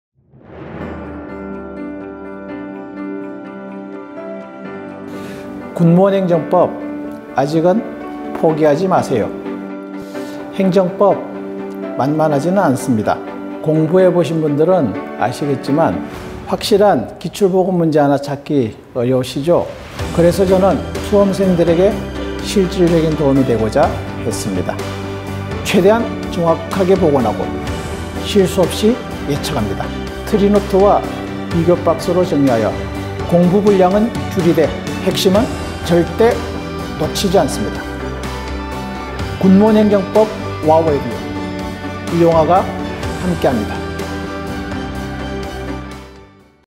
와우에듀 군무원 행정법 마무리 LIVE 핵심특강
2019 군무원 하반기 채용시험 대비
군무원 마무리 LIVE 핵심특강

와우에듀 군무원 행정법 이영화 교수님
과 함께
군무원 시험 합격을 대비하세요!

12월 16일 월요일부터 19일 목요일까지
매일 저녁 7시 30분, 군무원 교수님들의 라이브 특강! 본방사수!